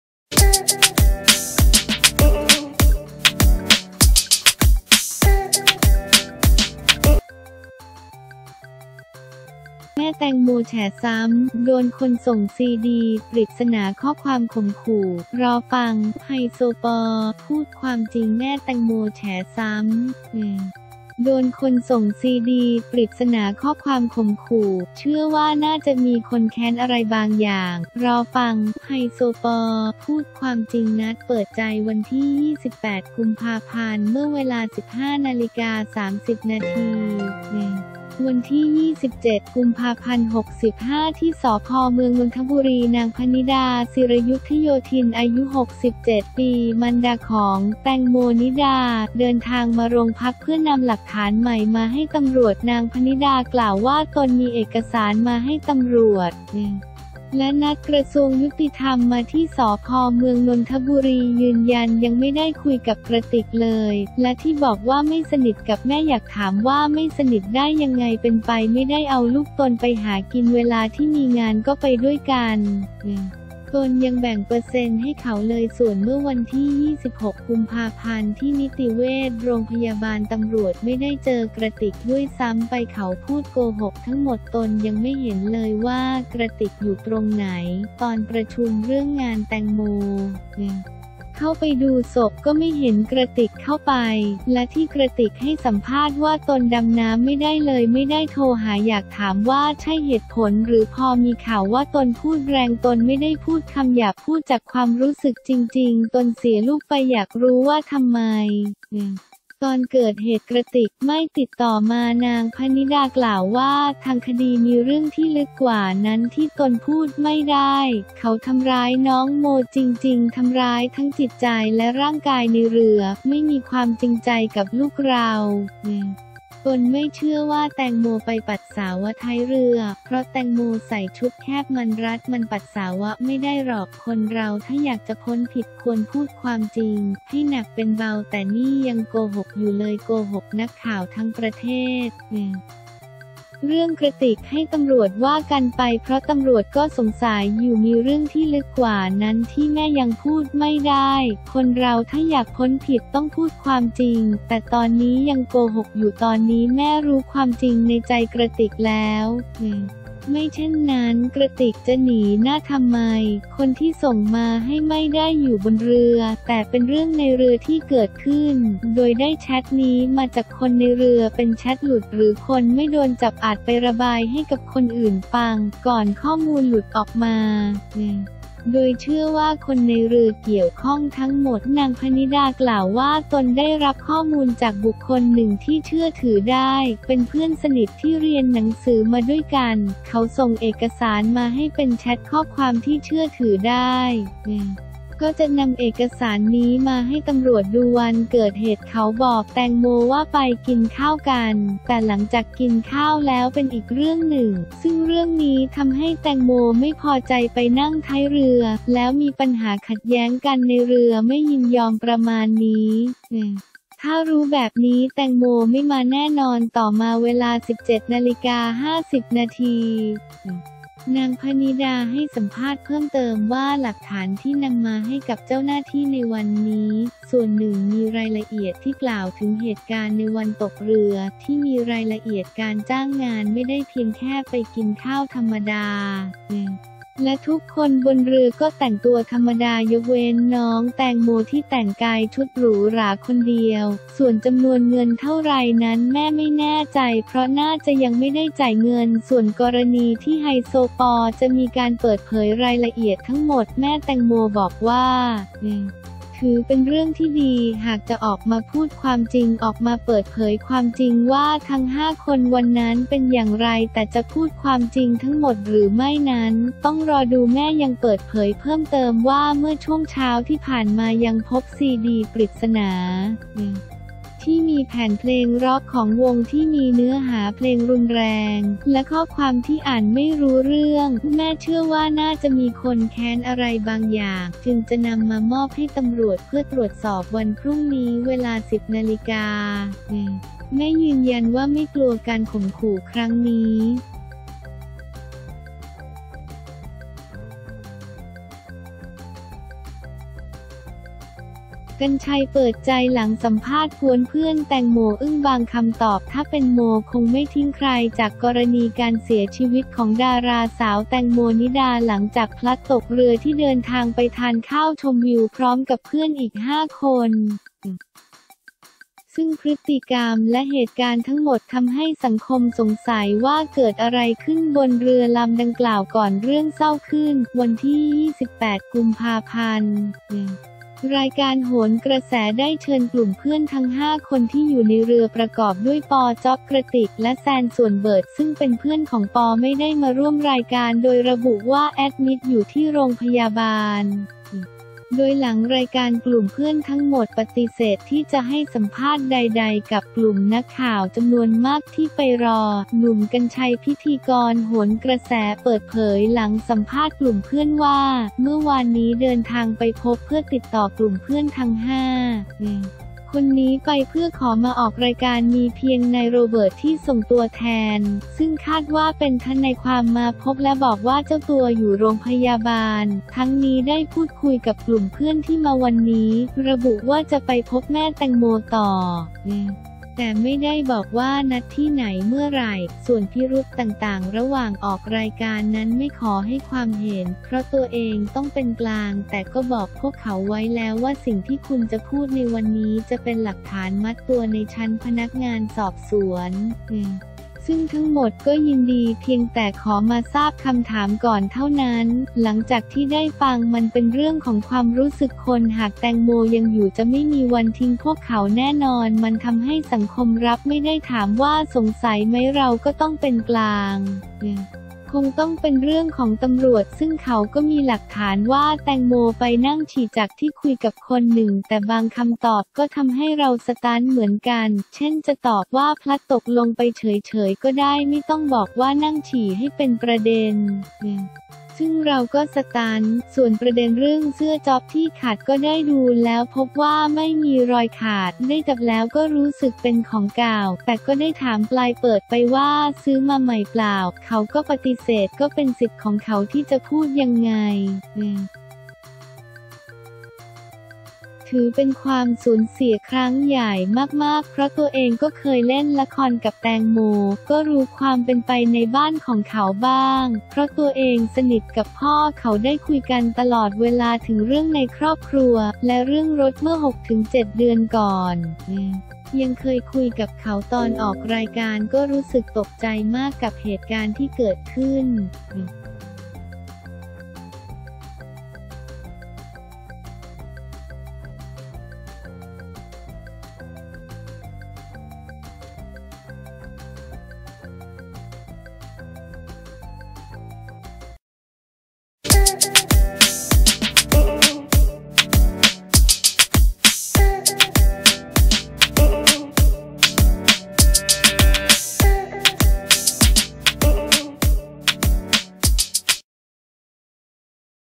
แม่แตงโมแฉซ้ำ! โดนคนส่งซีดีปริศนา ข้อความข่มขู่ เชื่อว่าน่าจะมีคนแค้นอะไรบางอย่าง รอฟัง ‘ไฮโซปอ’ พูด
แม่แตงโมแฉซ้ำ! โดนคนส่งซีดีปริศนา-ข้อความข่มขู่ รอฟัง 'ไฮโซปอ' พูดความจริง
แม่แตงโมแฉซ้ำ! โดนคนส่งซีดีปริศนา-ข้อความข่มขู่ เชื่อว่าน่าจะมีคนแค้นอะไรบางอย่าง รอฟัง ‘ไฮโซปอ’ พูดความจริง นัดเปิดใจ 28 ก.พ.
เมื่อเวลา 15.30 น. วันที่ 27 ก.พ.65 ที่ สภ.เมืองนนทบุรี นางภนิดา ศิระยุทธโยธิน อายุ 67 ปี มารดาของ ‘แตงโม นิดา’ เดินทางมาโรงพัก เพื่อนำหลักฐานใหม่ มาให้ตำรวจ
นางภนิดา กล่าวว่า ตนมีเอกสารมาให้ตำรวจ และนัดกระทรวงยุติธรรมมาที่ สภ.เมืองนนทบุรี ยืนยันยังไม่ได้คุยกับกระติกเลย และที่บอกว่าไม่สนิทกับแม่ อยากถามว่าไม่สนิทได้ยังไง เป็นไปไม่ได้ เอาลูกตนไปหากิน เวลาที่มีงานก็ไปด้วยกัน ตนยังแบ่งเปอร์เซ็นต์ให้เขาเลย
ส่วนเมื่อวันที่ 26 ก.พ.ที่นิติเวชโรงพยาบาลตำรวจ ไม่ได้เจอกระติกด้วยซ้ำไป เขาพูดโกหกทั้งหมด ตนยังไม่เห็นเลยว่ากระติกอยู่ตรงไหน ตอนประชุมเรื่องงานแตงโม เข้าไปดูศพก็ไม่เห็นกระติกเข้าไป และที่กระติกให้สัมภาษณ์ว่า ตนดำน้ำไม่ได้เลยไม่ได้โทรหา อยากถามว่าใช่เหตุผลหรือ พอมีข่าวว่าตนพูดแรง ตนไม่ได้พูดคำหยาบ พูดจากความรู้สึกจริงๆ ตนเสียลูกไป อยากรู้ว่าทำไม ตอนเกิดเหตุกระติกไม่ติดต่อมา

นางภนิดา กล่าวว่า ทางคดีมีเรื่องที่ลึกกว่านั้นที่ตนพูดไม่ได้ เขาทำร้ายน้องโมจริงๆ ทำร้ายทั้งจิตใจและร่างกายในเรือ ไม่มีความจริงใจกับลูกเรา ตนไม่เชื่อว่าแตงโมไปปัสสาวะท้ายเรือ เพราะแตงโมใส่ชุดแคบ มันรัด มันปัสสาวะไม่ได้หรอก คนเราถ้าอยากจะพ้นผิดควรพูดความจริง ให้หนักเป็นเบา แต่นี่ยังโกหกอยู่เลย โกหกนักข่าวทั้งประเทศ
“เรื่องกระติกให้ตำรวจว่ากันไป เพราะตำรวจก็สงสัยอยู่ มีเรื่องที่ลึกกว่านั้นที่แม่ยังพูดไม่ได้ คนเราถ้าอยากพ้นผิดต้องพูดความจริง แต่ตอนนี้ยังโกหกอยู่ ตอนนี้แม่รู้ความจริงในใจกระติกแล้ว ไม่เช่นนั้นกระติกจะหนีหน้าทำไม